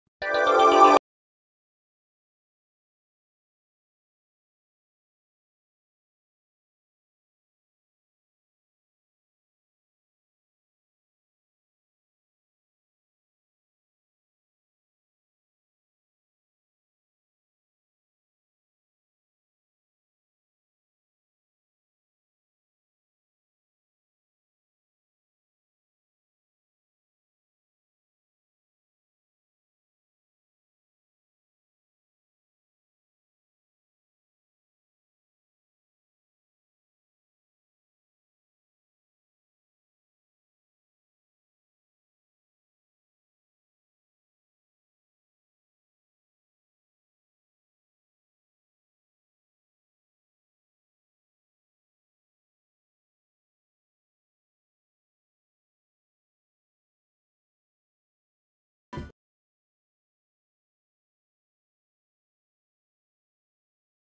The ASTRO-H space observatory: New insights into the hot universe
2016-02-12 - The Universe... it appears to be a vast, cold, quiet expanse of space. But is it? X-ray astronomy satellite ASTRO-H will take flight to unravel the hot, violent Universe hidden deep in the night sky.